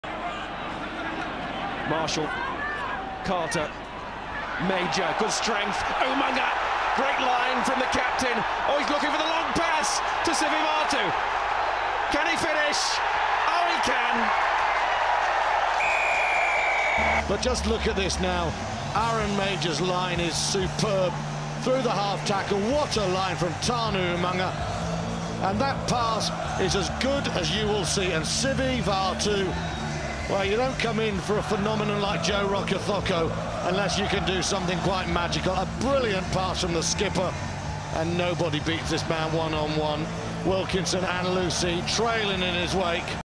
Tana Umaga breathtaking pass for Sitiveni Sivivatu
Aaron Mauger punches a hole through the British & Irish Lions defence he then offloads to the New Zealand captain Tana Umaga who gives an incredible pass for winger Sitiveni Sivivatu to run in and beat the last defender to score a try during the All Blacks 21-3 win over the Lions in Christchurch in the first test of New Zealand's 3-0 series win.